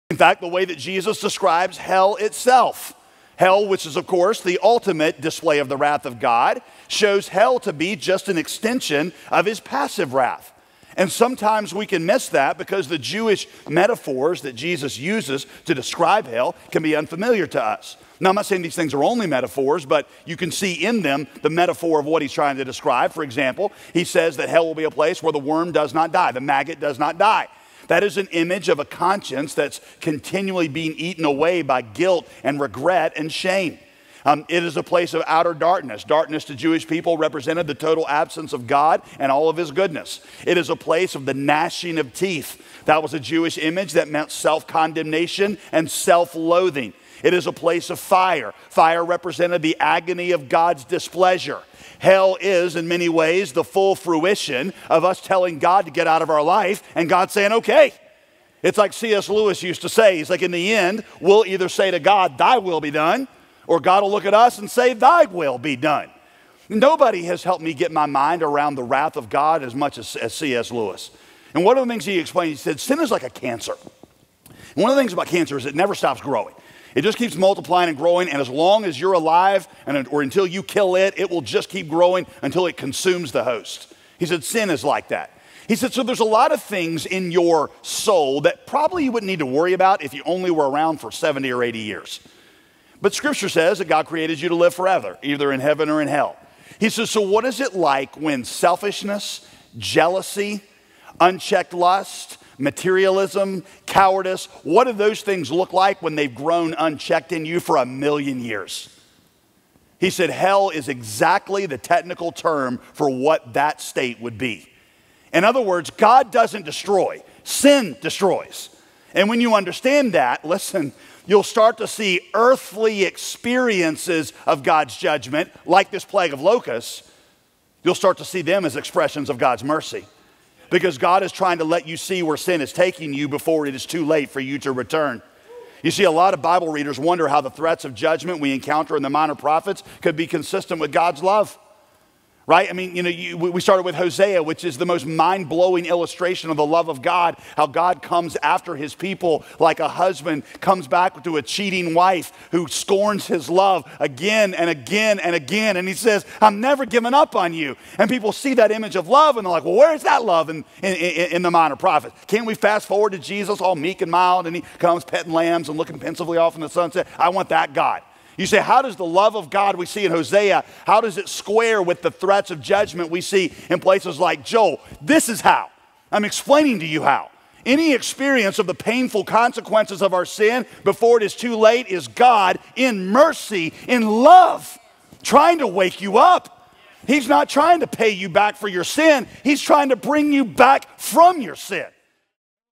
Bringing You Back From Your Sin
God isnt trying to pay you back for your sin he is trying to bring you back from your sin.